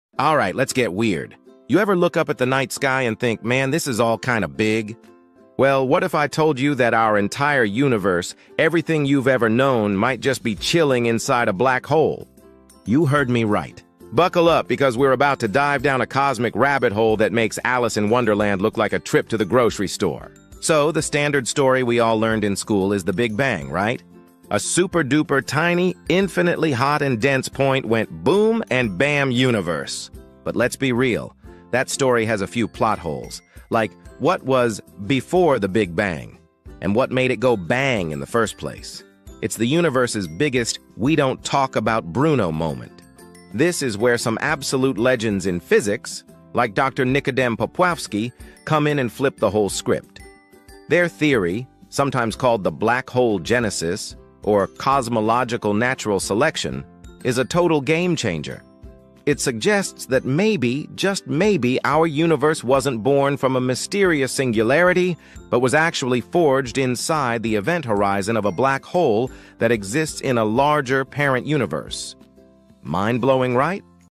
Is Our Universe Inside a Black Hole?!
Join us as we delve into one of the most mind-bending theories in modern astrophysics: the idea that our universe could be inside a black hole. This concept, although sounding like science fiction, has garnered significant attention and debate among physicists and cosmologists. The theory suggests that our universe is a three-dimensional brane, or membrane, floating in a higher-dimensional space called the "bulk." One possibility is that this brane is inside a black hole in the bulk, which would explain some of the observed features of our universe, such as its expansion and the existence of dark matter and dark energy. In this video, we'll explore the theoretical framework behind this idea, discussing the implications of being inside a black hole and what it could mean for our understanding of the cosmos and the laws of physics. We'll examine the potential evidence for and against this theory, and consider what it might imply about the nature of reality itself. Is our universe indeed nested inside a black hole, and if so, what are the consequences for our understanding of space, time, and gravity? Let's dive into the fascinating and complex world of cosmology and theoretical physics to explore this extraordinary concept.